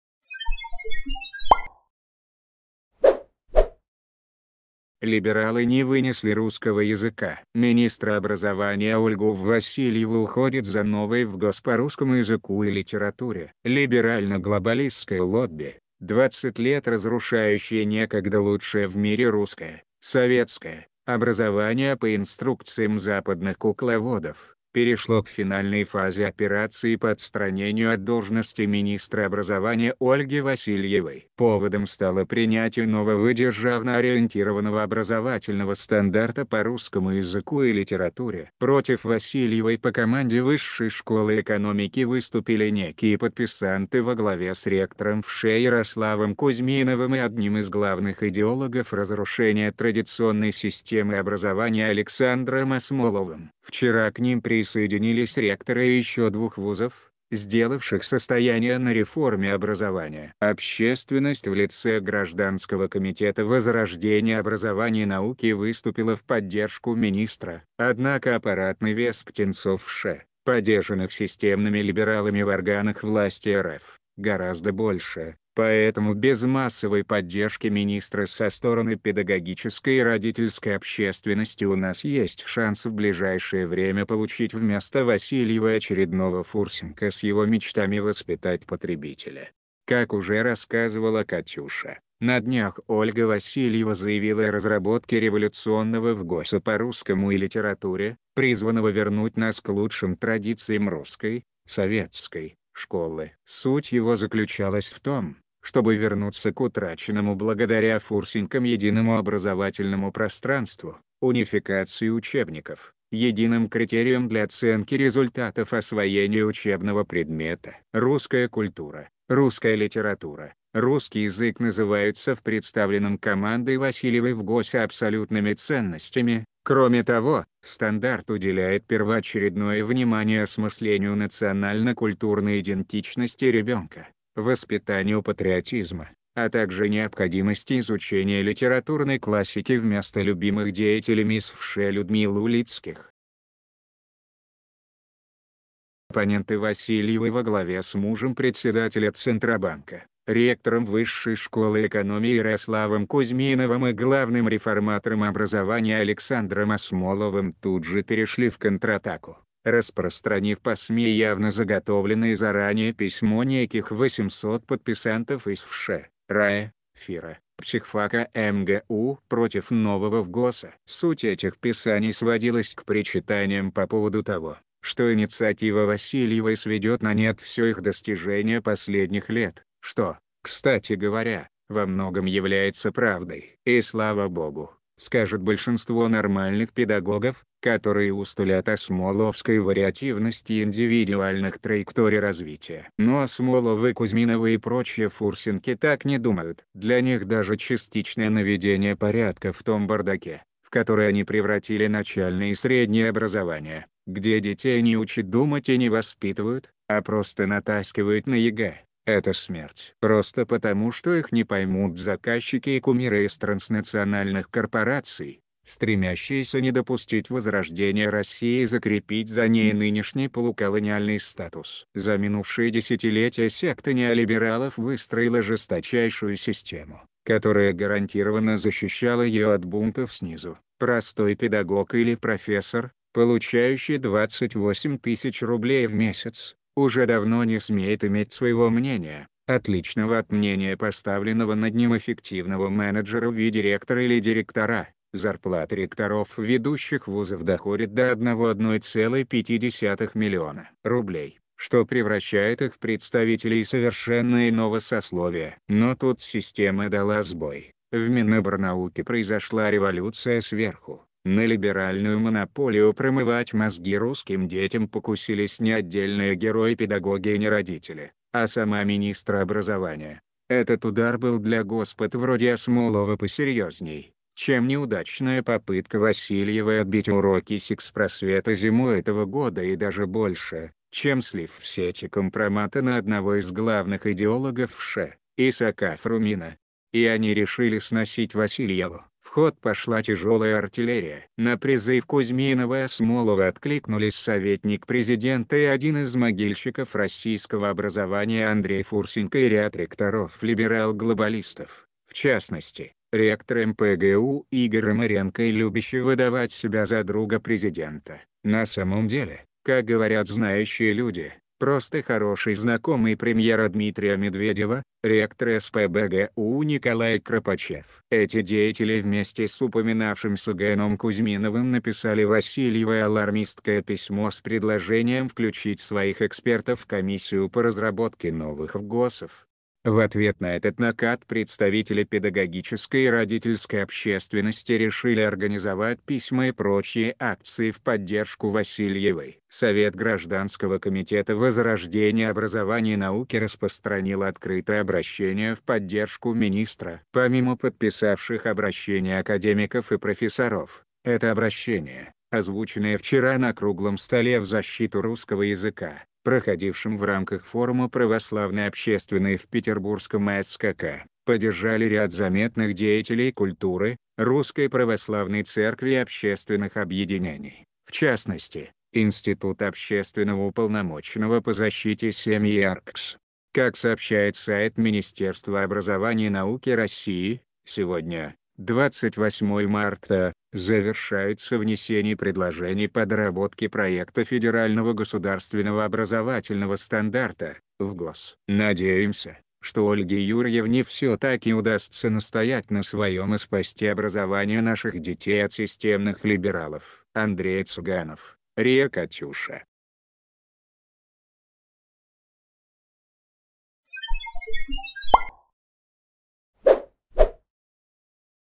Министра образования Ольгу Васильеву «уходят» за новый ФГОС по русскому языку и литературе
Либерально-глобалистское лобби, 20 лет разрушающее некогда лучшее в мире русское (советское) образование по инструкциям западных кукловодов, перешло к финальной фазе операции по отстранению от должности министра образования Ольги Васильевой. Поводом стало принятие нового державно-ориентированного образовательного стандарта по русскому языку и литературе. Против Васильевой по команде Высшей школы экономики выступили некие подписанты во главе с ректором ВШЭ Ярославом Кузьминовым и одним из главных идеологов разрушения традиционной системы образования Александром Асмоловым. Вчера к ним присоединились ректоры еще двух вузов, сделавших состояния на реформе образования. Общественность в лице Гражданского комитета возрождения образования и науки выступила в поддержку министра. Однако аппаратный вес птенцов ВШЭ, подержанных системными либералами в органах власти РФ, гораздо больше, поэтому без массовой поддержки министра со стороны педагогической и родительской общественности у нас есть шанс в ближайшее время получить вместо Васильевой очередного Фурсенко с его мечтами «воспитать потребителя».